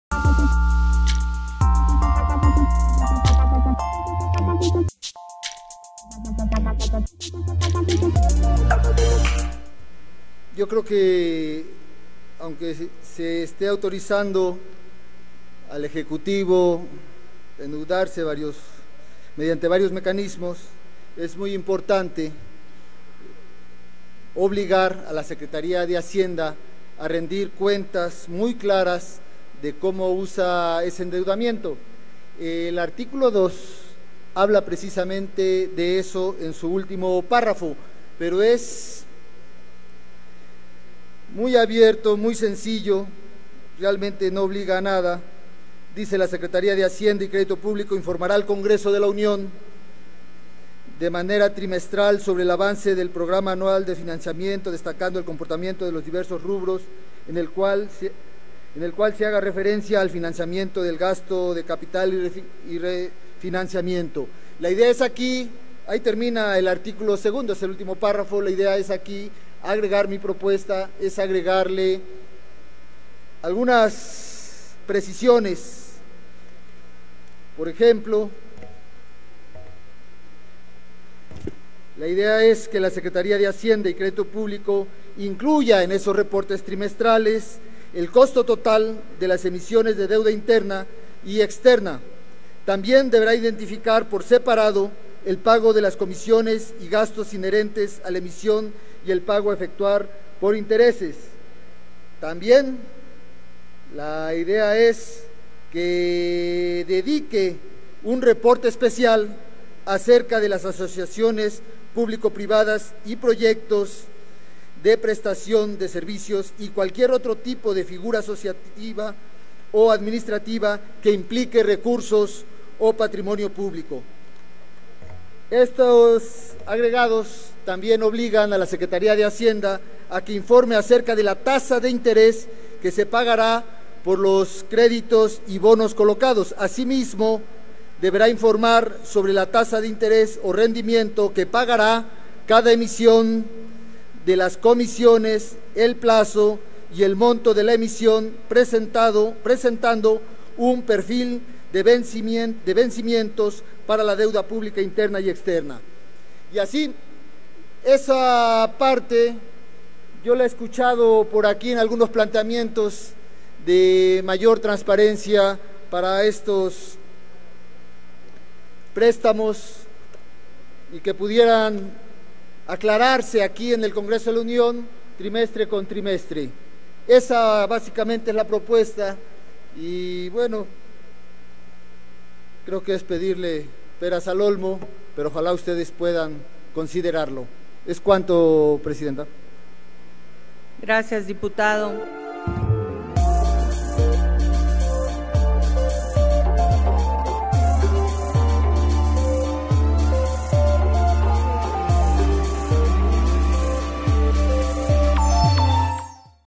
Dip. Cuitláhuac García (MORENA) - Ley de Ingresos 2017 (Reserva)
El Dip. Cuitláhuac García Jiménez (MORENA) refiere la importancia de obligar a la SHCP a informar sobre el uso de endeudamiento e incluir el costo total de deuda interna y externa.